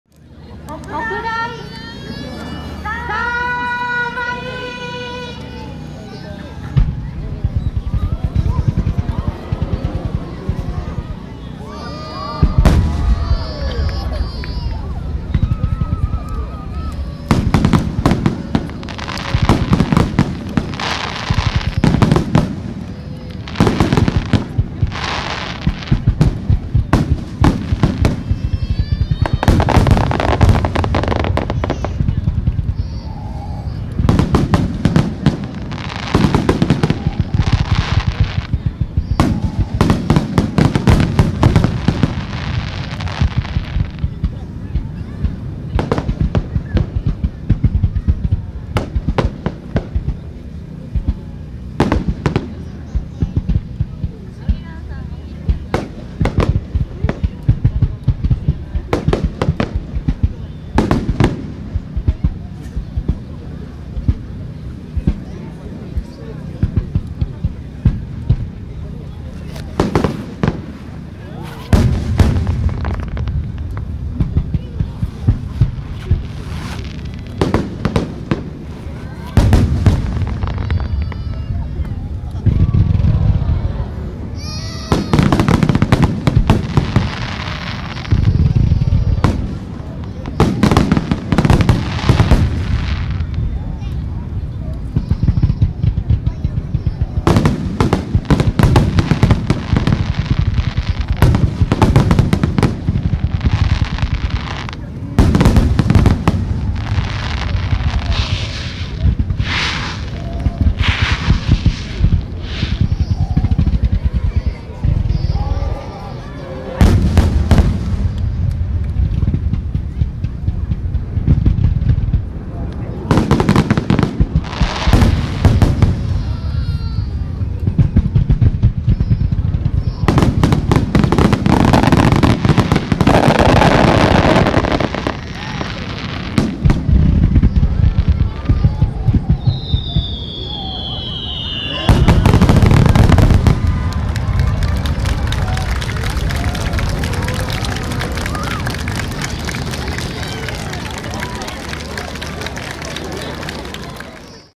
2022.7.23 安倍川花火大会 フィナーレ特大スターマイン
特大スターマイン(お客様の暮らしをデザインし提案する「ライフデザイングループ」TOKAIグループ提供)
4年連続中止で5年ぶり69回目の安倍川花火大会
今年は例年の2時間から1時間に短縮し 打ち上げ場所も800mほど上流で開催された
次回は本部近くの正面から撮影したい